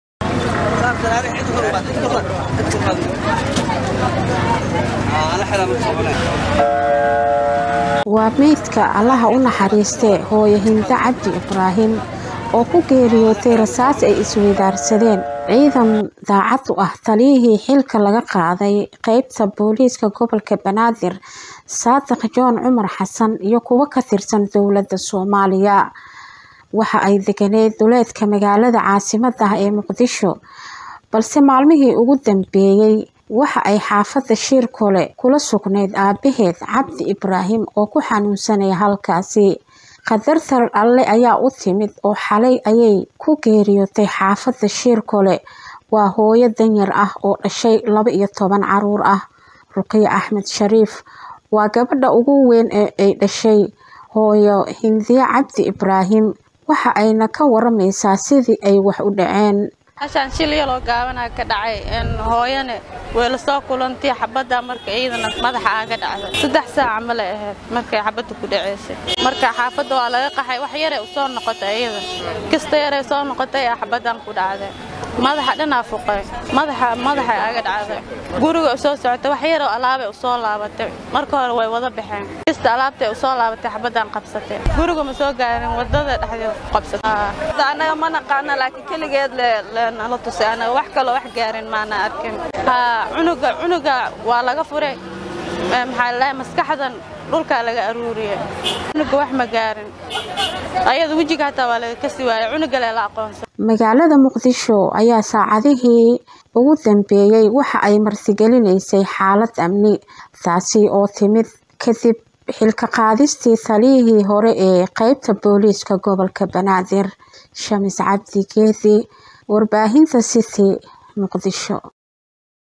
HOOYO HINDIYO OO U GEERYOOTAY RISAAS AY ISWEEDAARSADEEN CIIDAMO DOWLADA KATIRSAN IYO
HOOYO HINDIYO OO U GEERYOOTAY RISAAS AY ISWEEDAARSADEEN CIIDAMO DOWLADA KATIRSAN IYO KUWO DAACAD U AH JOON BY SHAMIS CABDI GEEDI